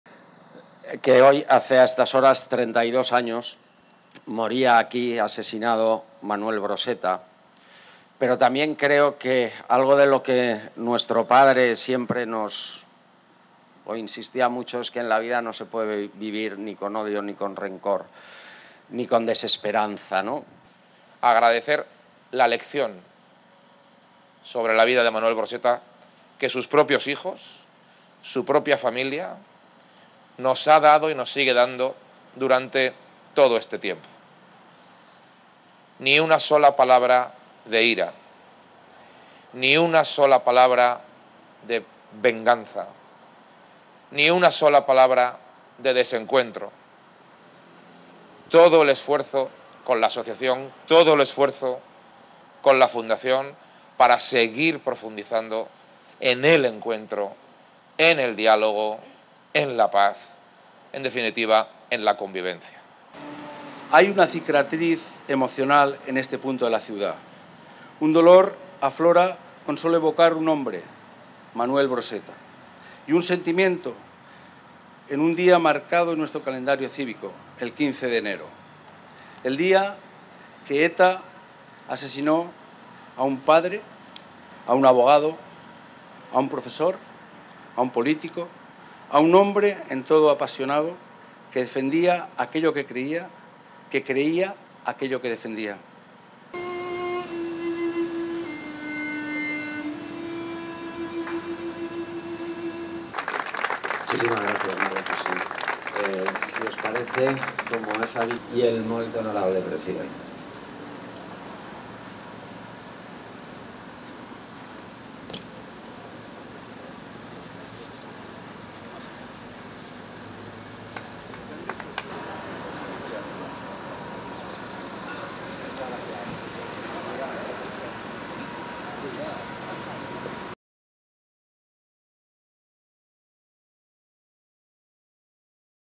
Mazón y Puig homenajean al profesor Broseta en el aniversario de su asesinato por ETA en Valencia
Su hijo pide que no haya gobiernos «con miembros con delitos de sangre a sus espaldas» y Mazón defiende que «sin libertad no hay democracia»

Representantes de las instituciones valencianas y de la sociedad civil se han sumado al tradicional homenaje al profesor Manuel Broseta con motivo del 32 aniversario de su asesinato a manos de ETA, un acto en el que su hijo, Pablo Broseta, ha instado a aprender de su padre que «en la vida no se puede vivir ni con odio, ni con rencor, ni con desesperanza».

Vídeo: EP